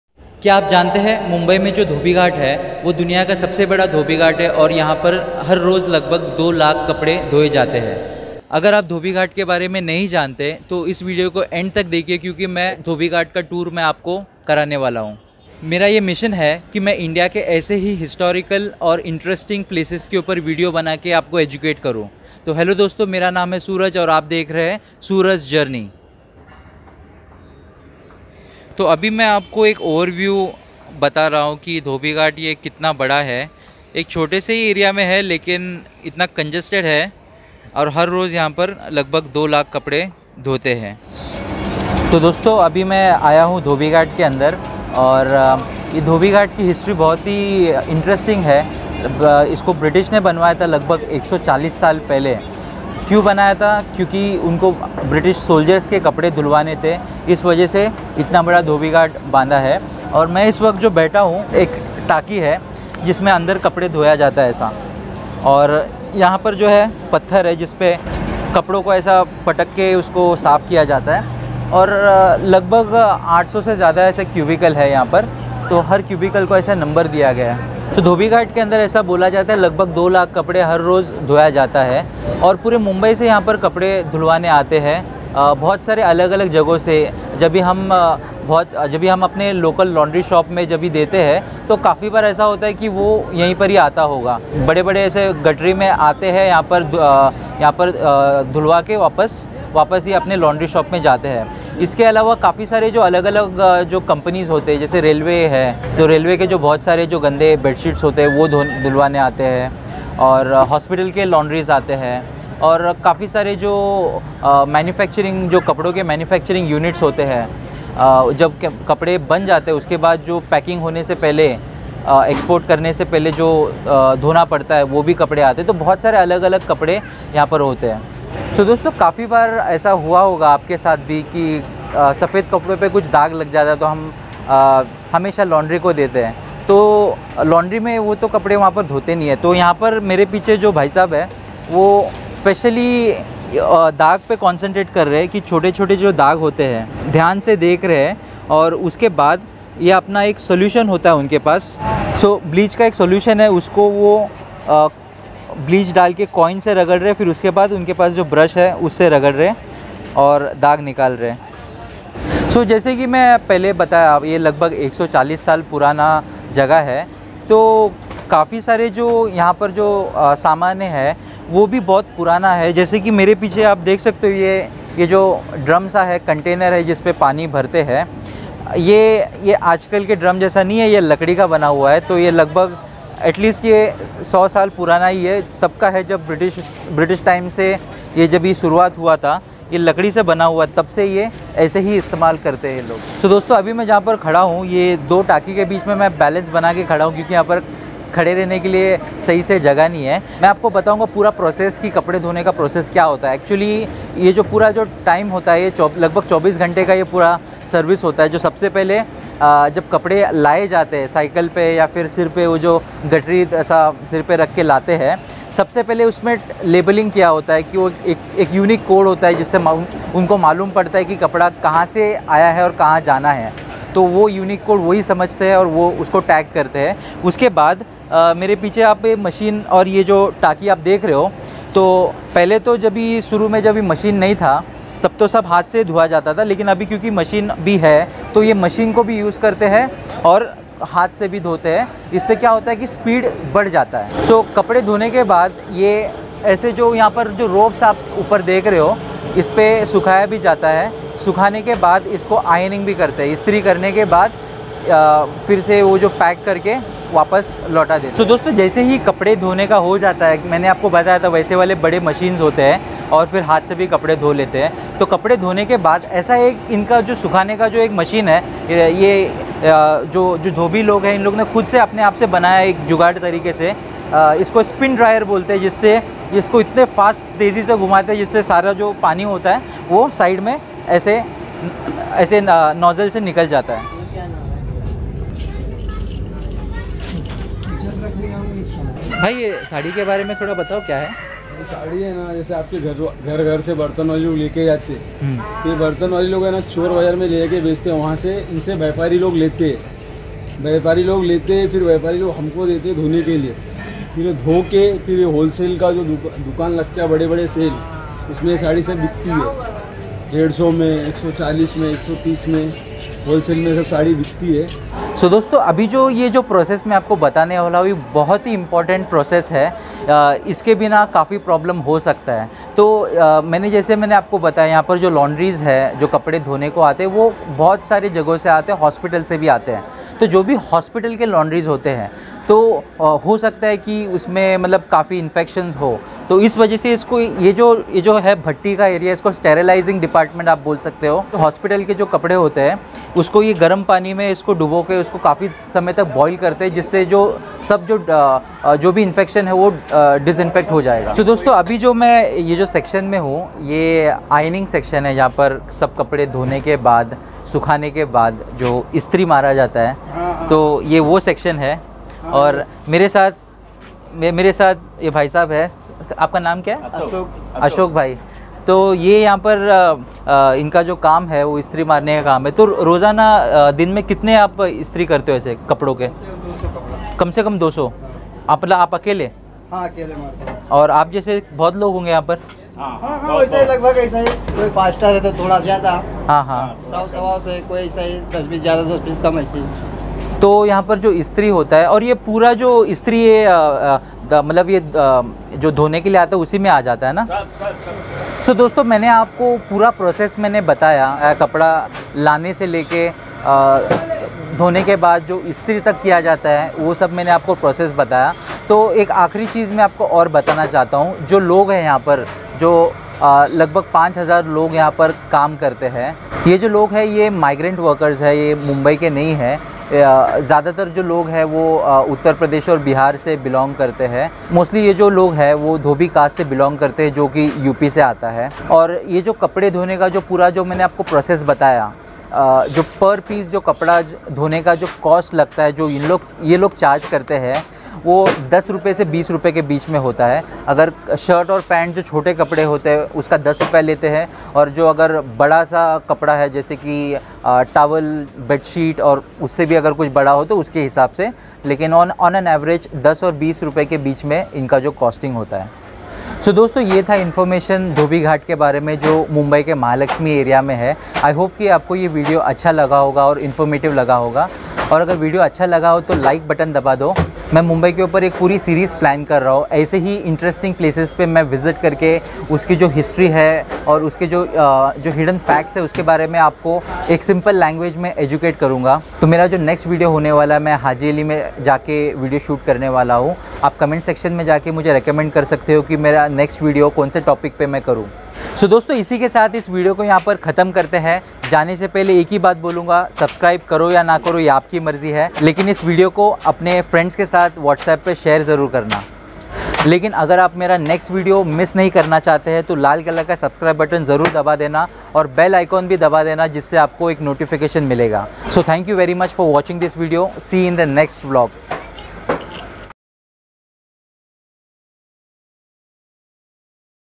Dhobi Ghat Documentary| Inside the Biggest laundry in the world.
In this video I visit the biggest laundry in the world which is in Mahalaxmi (Mumbai).
You will get to understand the entire information in detail.